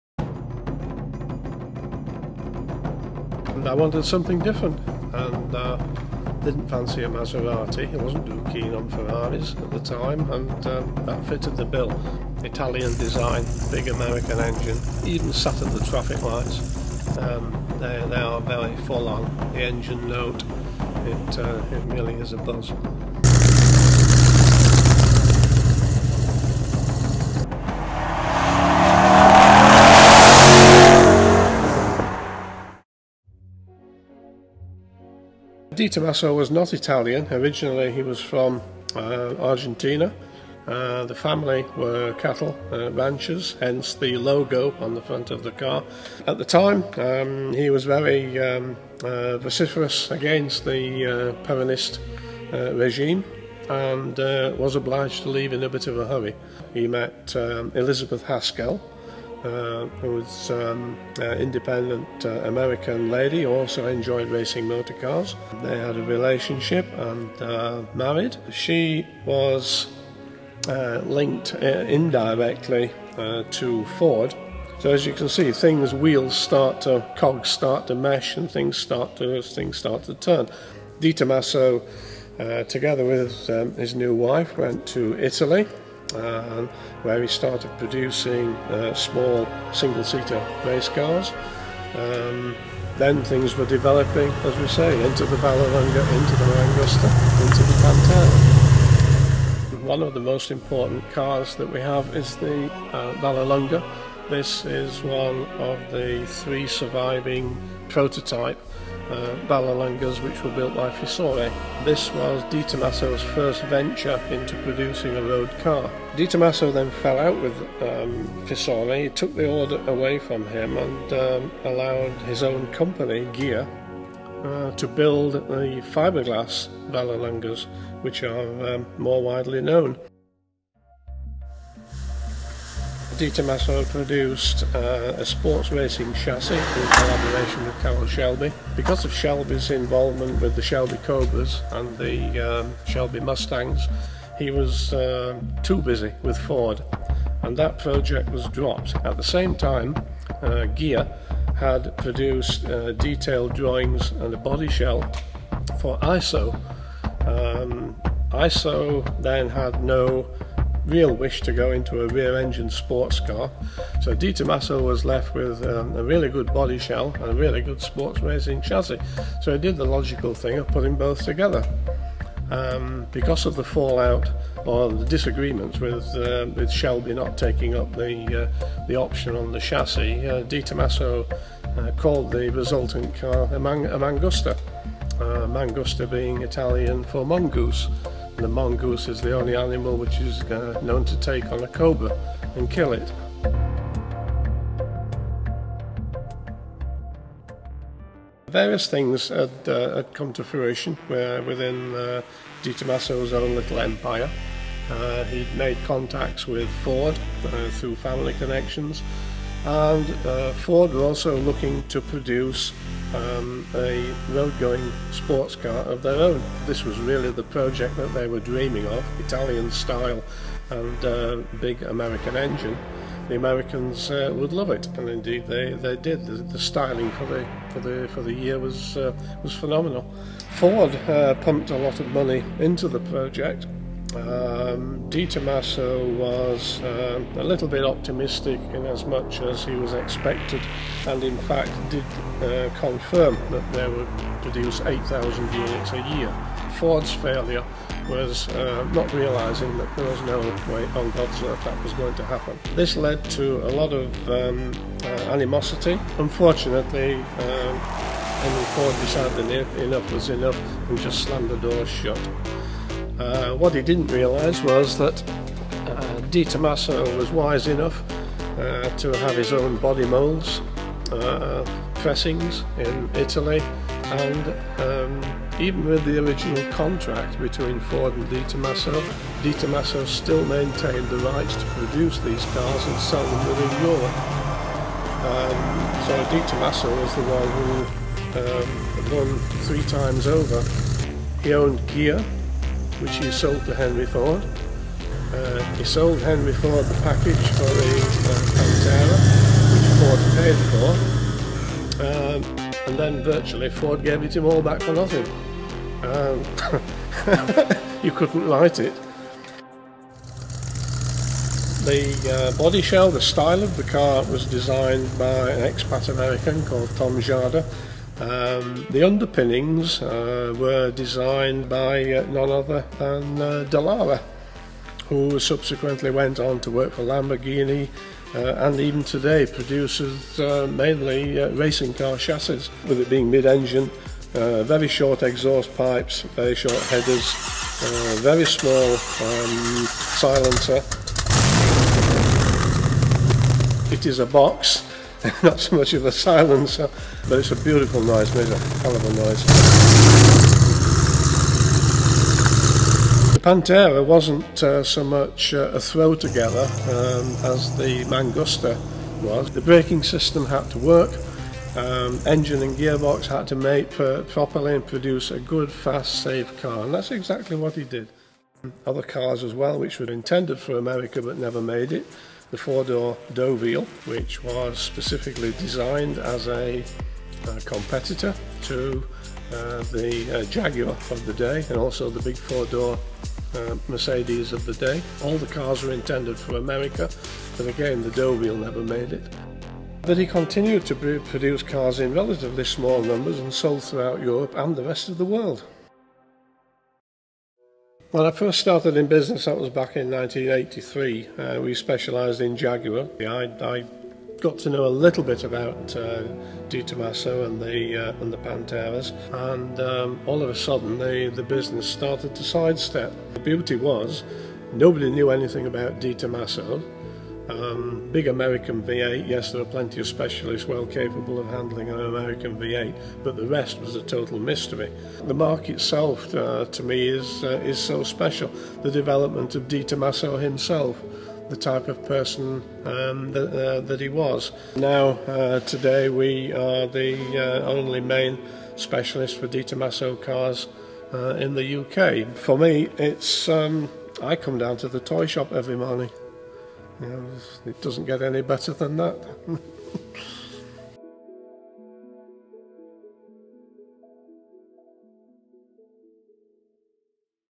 The Beautiful History of De Tomaso - /DRIVEN
De Tomaso is most known for designing the stunner that was the Pantera, but  that wasn't the only winner.